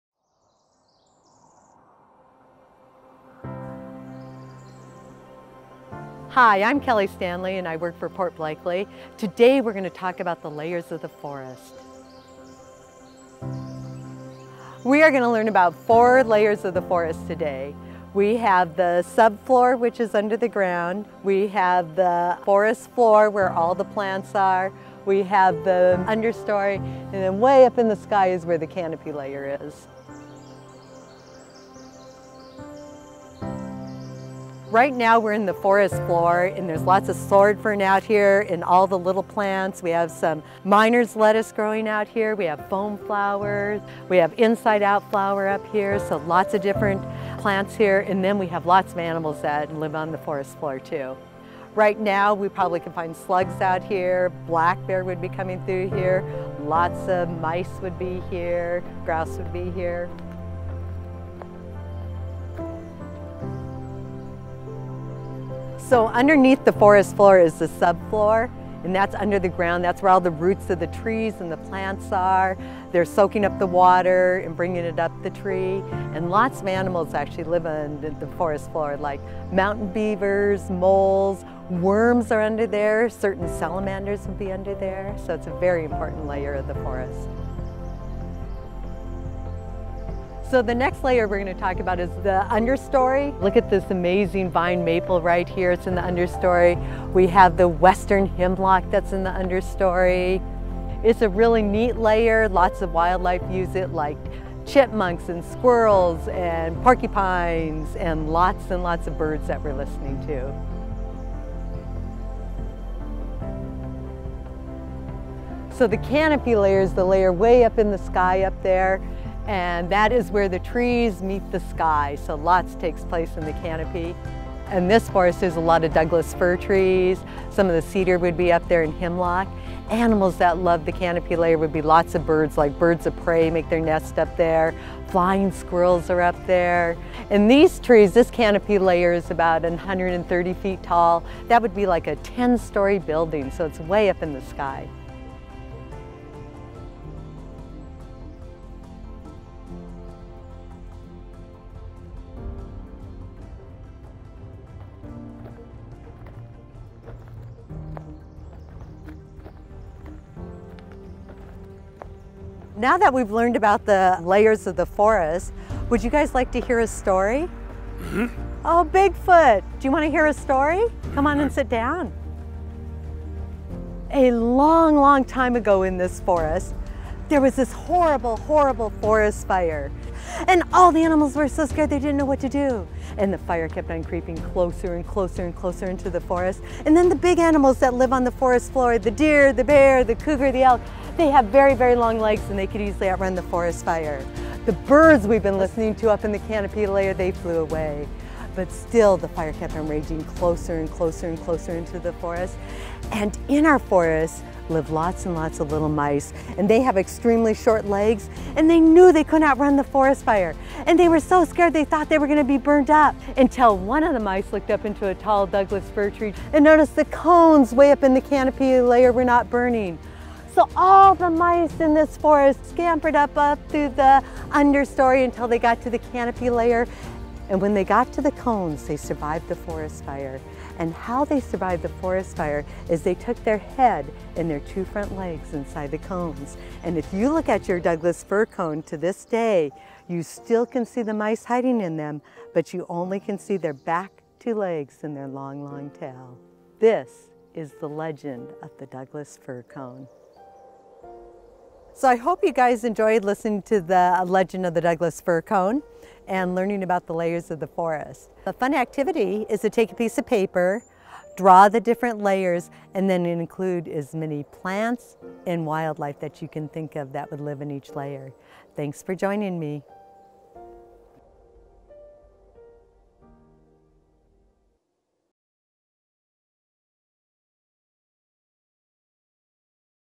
Layers of a Forest
Did you know there are four layers in a forest, from the forest floor to the canopy?  Watch the video to find out where all the critters in the forest live.